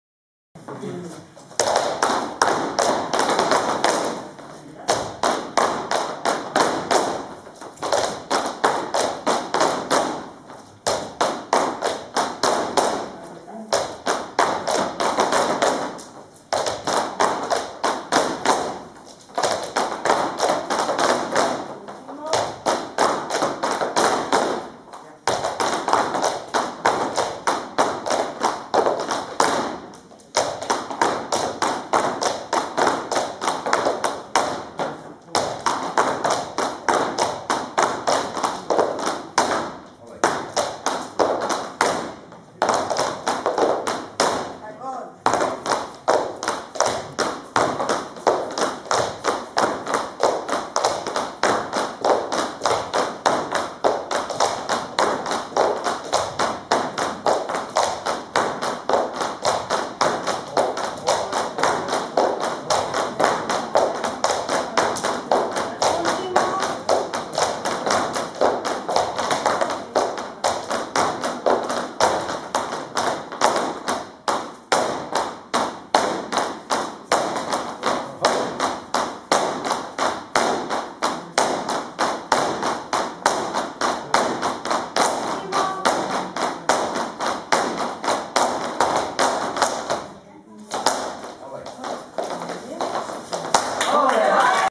Fragmento 14
Exhibición bailes de Sevillanas y Flamenco del Grupo de la Asociación Valorio-San Lázaro.